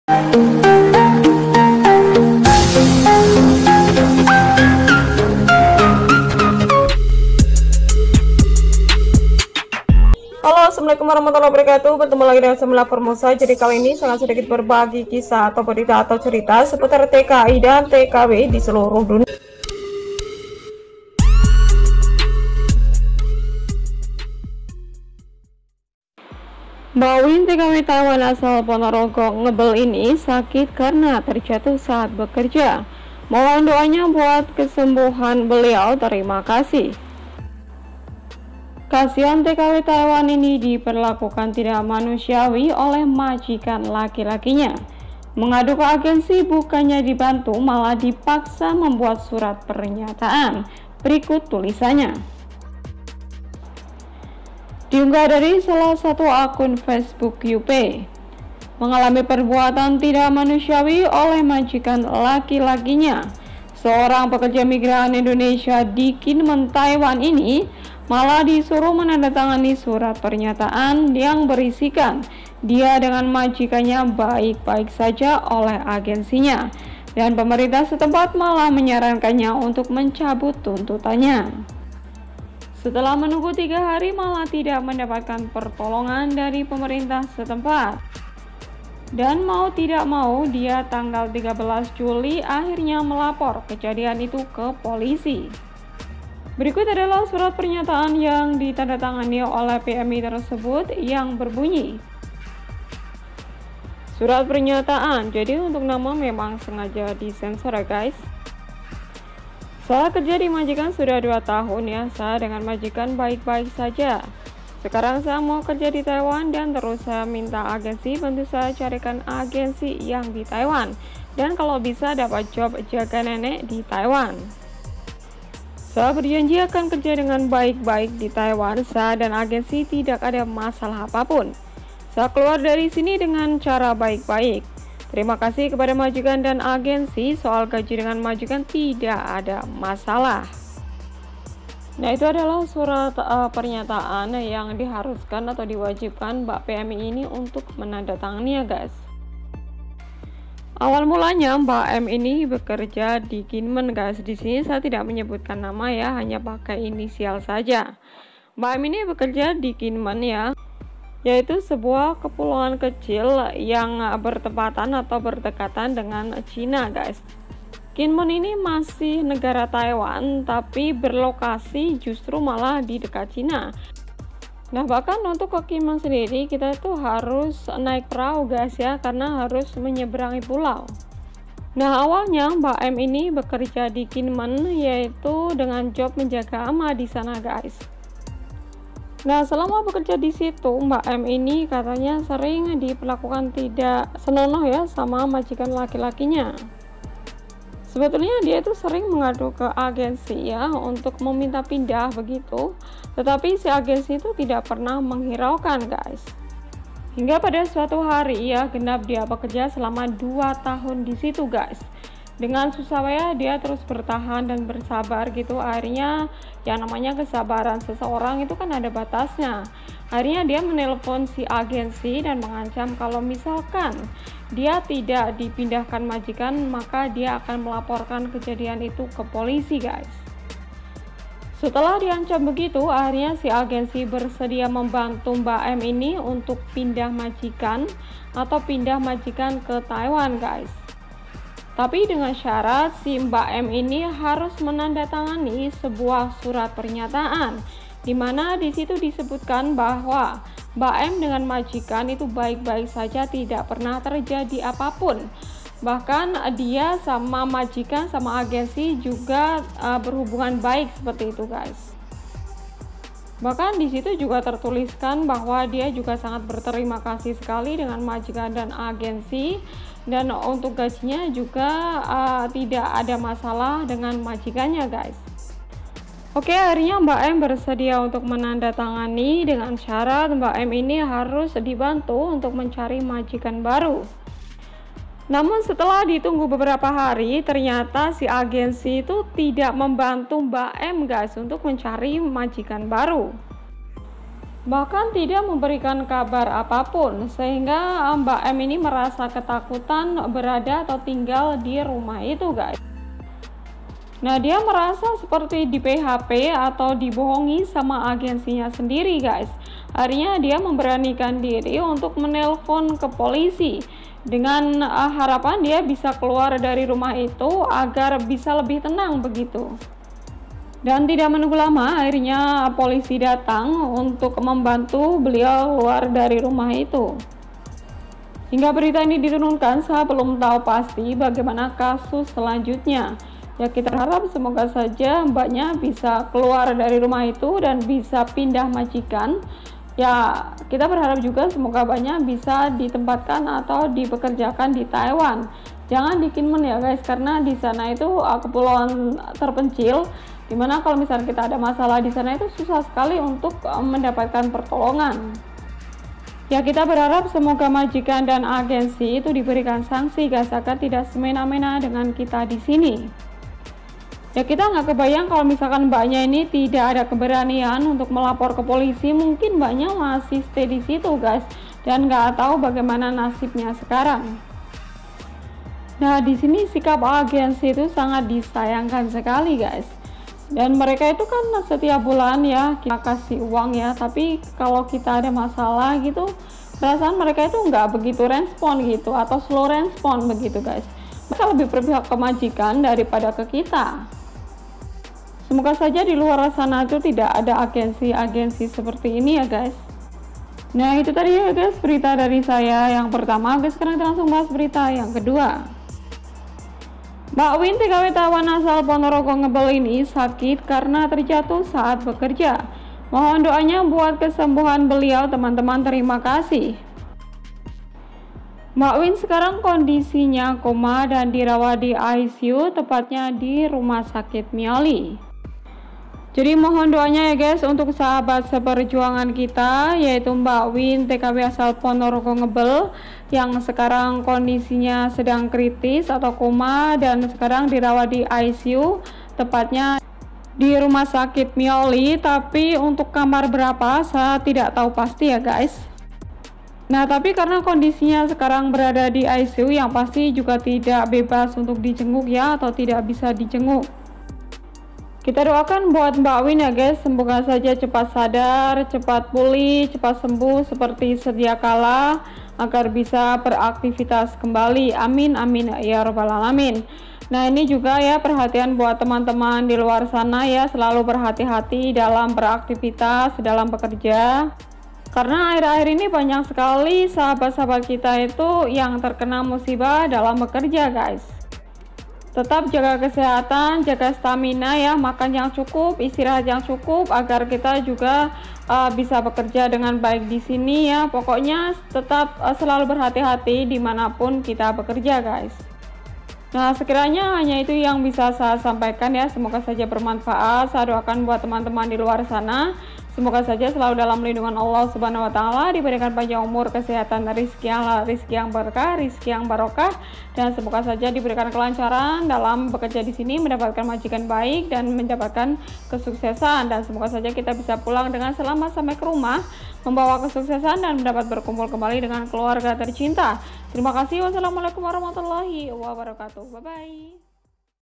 VIRAL!!TKW TAIWAN DI ANU MAJIKANYA.. MINTA PINDAH MALAH DI PAKSA AGENCY LAKUKAN HAL INI ..
ceritatkwtaiwan suarabmi liputan bmi#tkwtaiwan#tkwsingapore#tkwarabsaudi#tkwmalaysia#tkwhongkong#tkiarabsaudi#tkitaiwan#tkimalaysia#tkikorea#tkijepang#tkiarabsaudi#mbokcikrak#infoseputartkidantkw#milaformosa